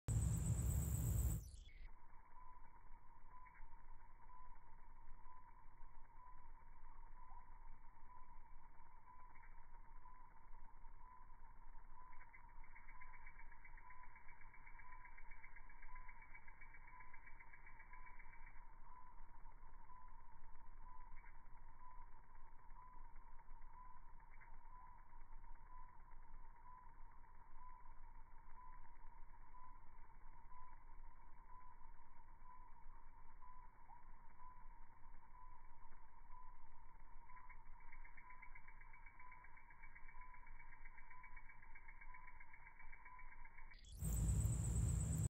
Hummingbird Moth In Slow Motion
Listen carefully... you can hear the wing beats!